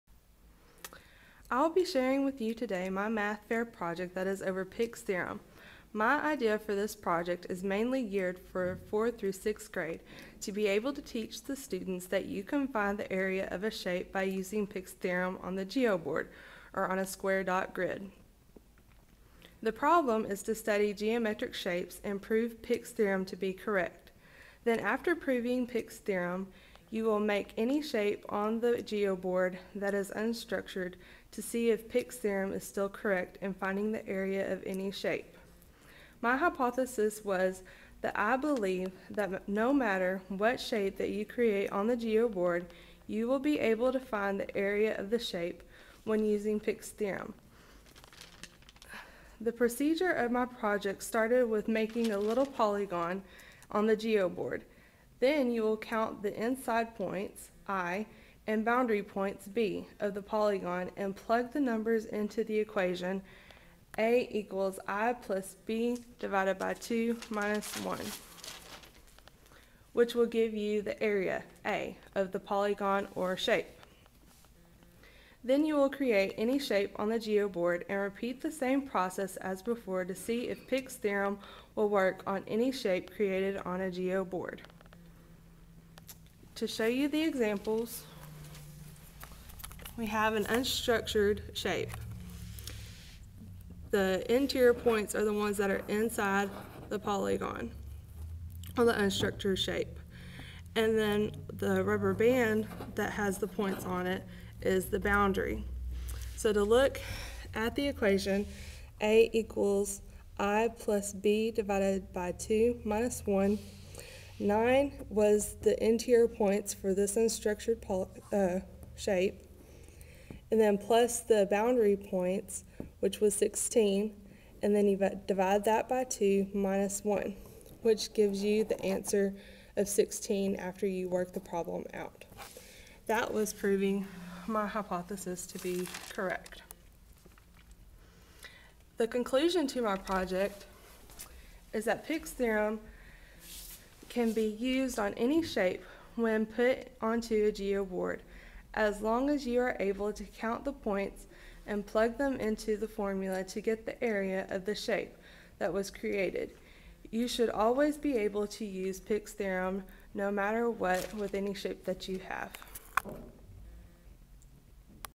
Pick's Theorem
Math Fair Project over Pick's Theorem- A brief explination